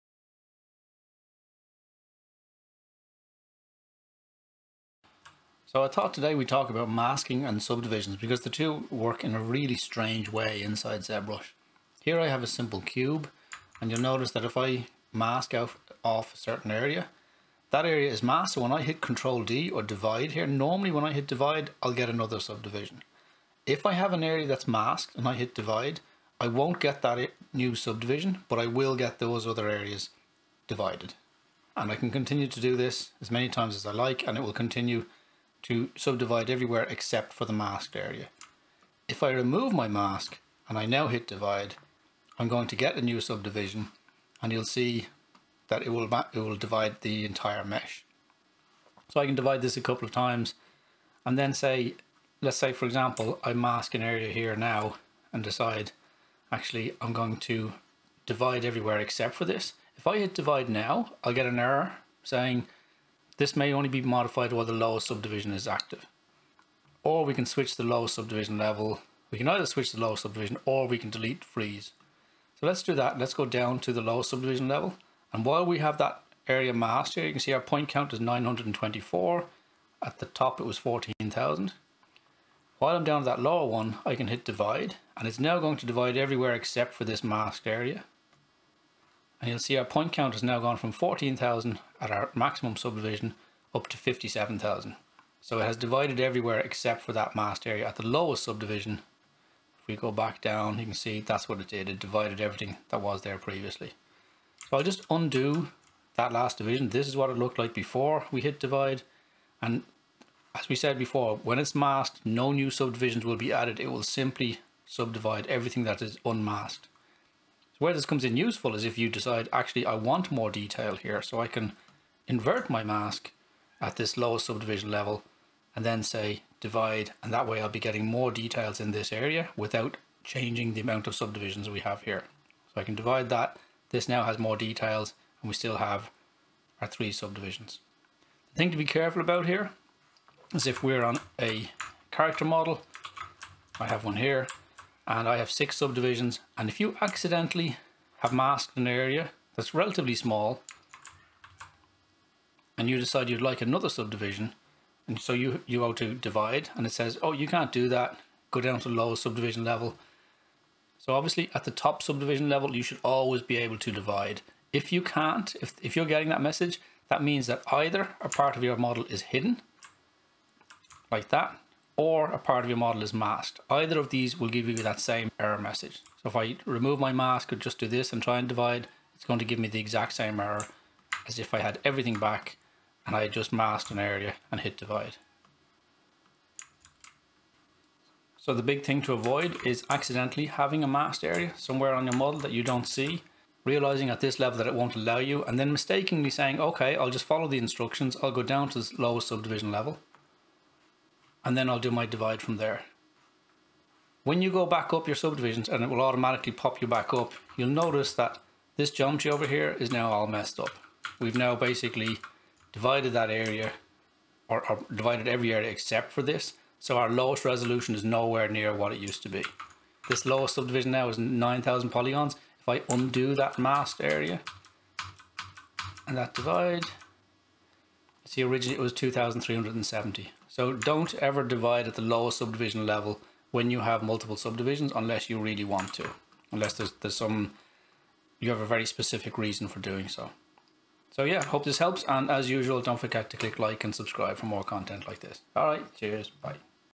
Using masks and subdivs in ZBrush - what to look out for!
It's easy to mess up your model if you don't use Masks and Subdivisions correctly in ZBrush, here's what to look out for.